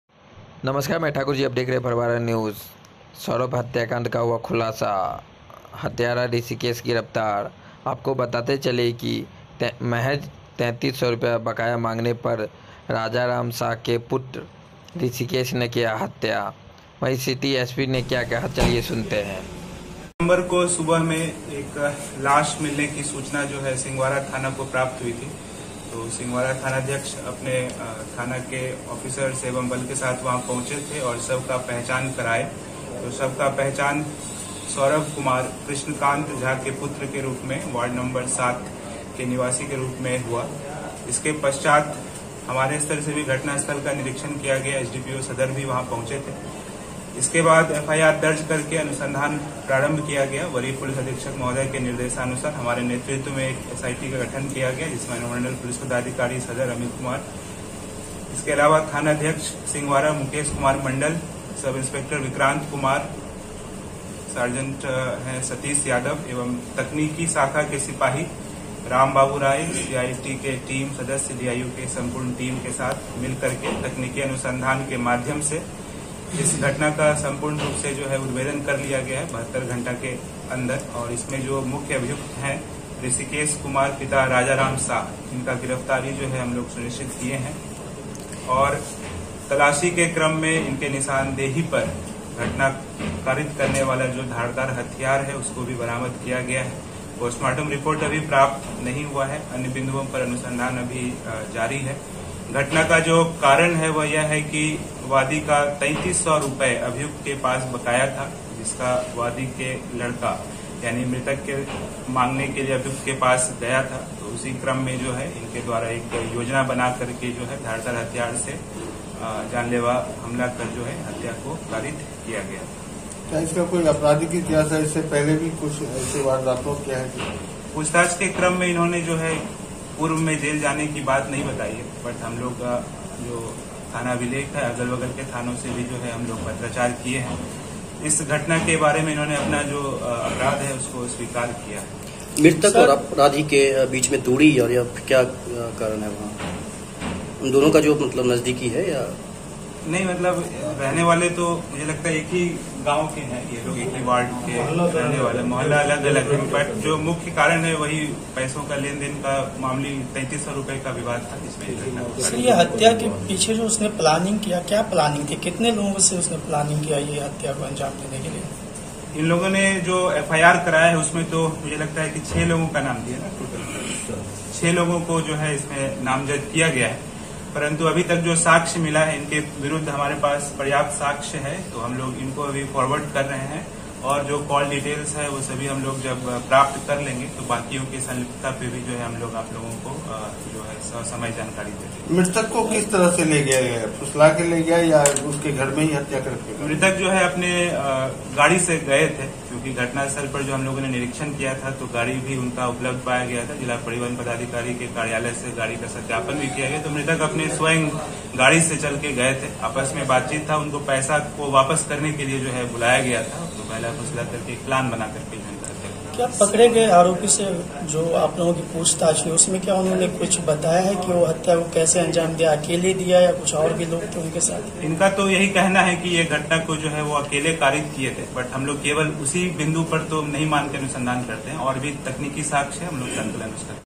सौरभ हत्याकांड का हुआ खुलासा,हत्यारा ऋषिकेश गिरफ्तार 3300 रुपए बकाया मांगने पर ऋषिकेश ने की हत्या
सौरभ झा हत्याकांड का हुआ खुलासा,हत्यारा ऋषिकेश गिरफ्तार

– महज 3300 रुपए बकाया मांगने पर ऋषिकेश ने की सौरभ की निर्मम हत्या